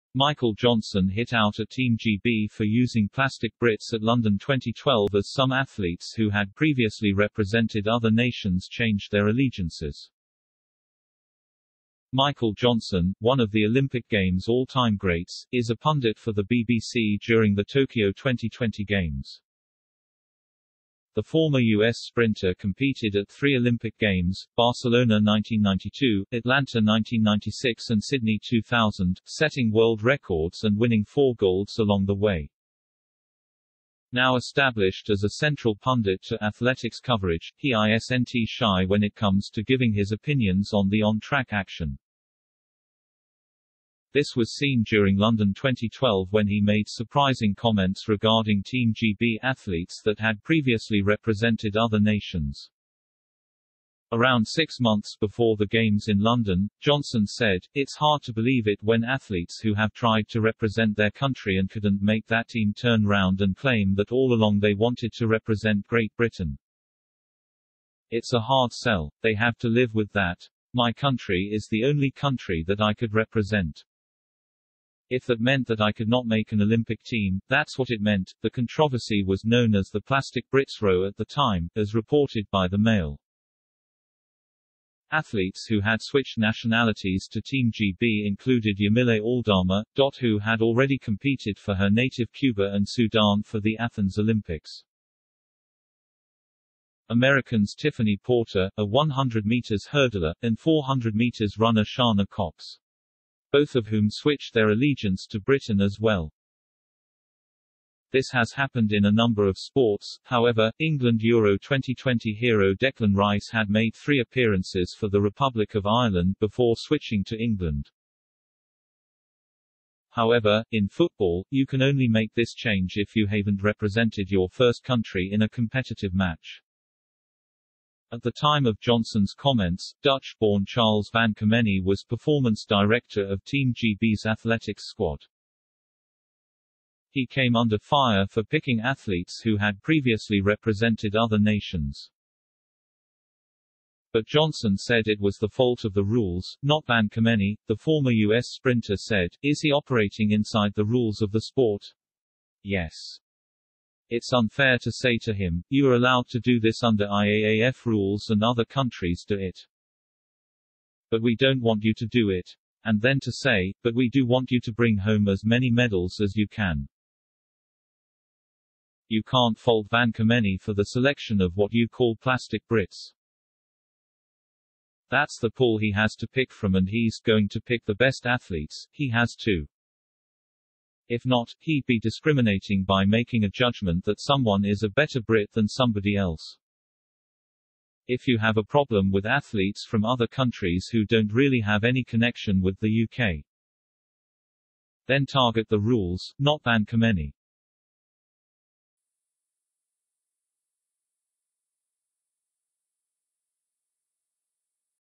Michael Johnson hit out at 'plastic Brits' in Olympics rant 'Have to live with that'
MICHAEL JOHNSON hit out at Team GB for using "plastic Brits" at London 2012 as some athletes who had previously represented other nations changed their allegiances.
MICHAEL JOHNSON#TeamGB#